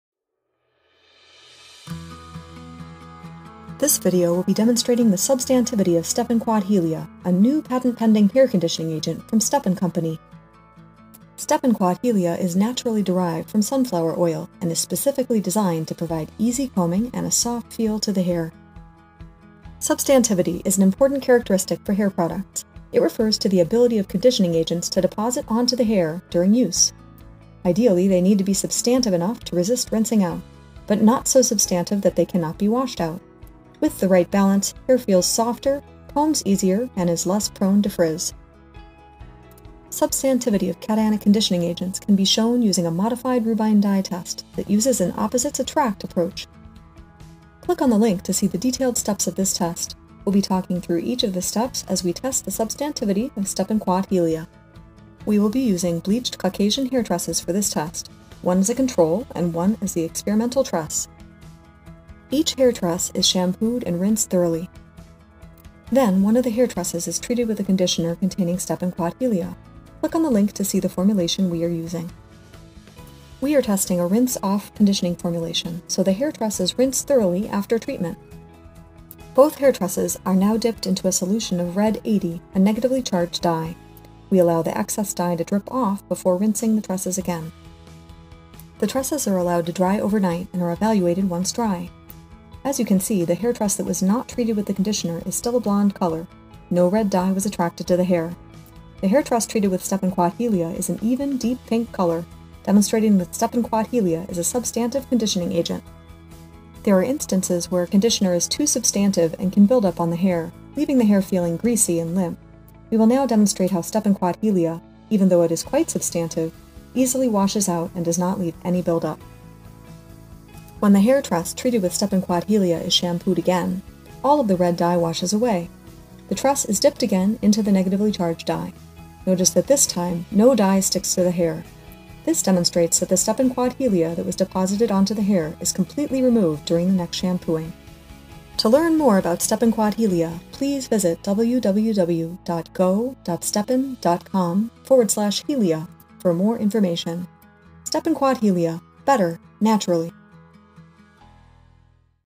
STEPANQUAT Helia Rubine Dye Test
This video demonstrates the substantivity of STEPANQUAT Helia®, a new patent-pending hair conditioning agent from Stepan Company. STEPANQUAT Helia is naturally-derived from sunflower oil and is specifically designed to provide easy combing and a soft feel to the hair.

Substantivity is an important characteristic for hair products. It refers to the ability of the conditioning agents to deposit onto the hair during use. Ideally, they need to be substantive enough to resist rinsing out, but not so substantive that they cannot be washed out. With the right balance, hair feels softer, combs easier, and is less prone to frizz.